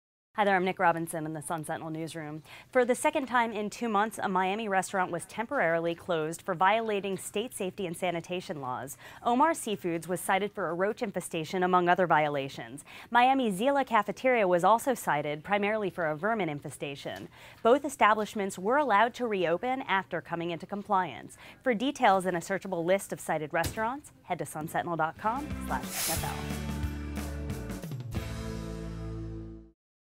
Miami Restaurant Cited for Roaches
WSFL News Update 02/08/12 6.59p Miami Restaurant Cited for Roaches